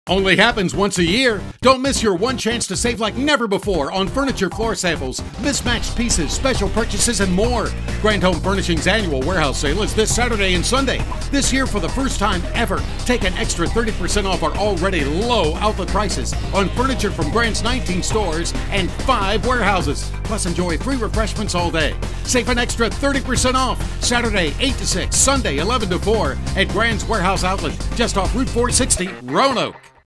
39th Annual Warehouse Sale
Saturday, February 20: 8am - 6pm
Sunday, February 21: 11am - 4pm

Once again, Grand Home Furnishings brings the state's biggest furniture sale to Roanoke! Save big on discontinued styles, overstocks and scratch & dent merchandise. Nearly $3 Million worth of furniture and mattresses to sell.

1221 Rhodes Avenue, Roanoke, VA 24012 Location Only